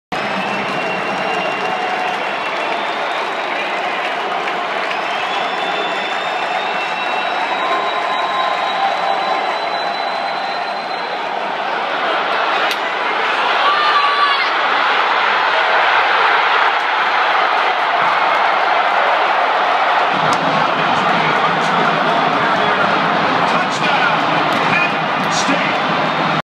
2012 MAC Football Championship - Kent State 15 Yd Rushing Touchdown
2012 MAC Football Championship - Northern Illinois vs. Kent State - Kent State Dri Archer rush for 15 yards Touchdown